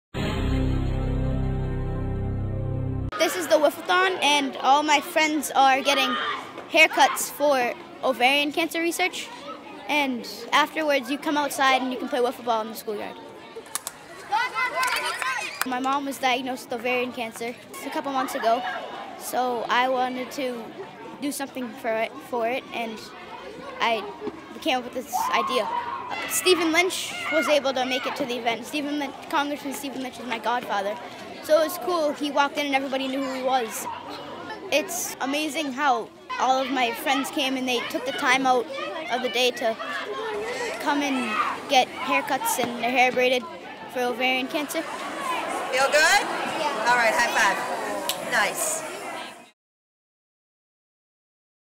Whifflethon to raise funds for cancer
South Boston's Brian Miller Jr. held a fund raiser to help raise funds for Ovarian Cancer Research after his mother , Lori, was diagnosed with the disease. The 11 year old who attends South Boston Catholic Academy and his godfather State Rep. Steve Lynch helped out.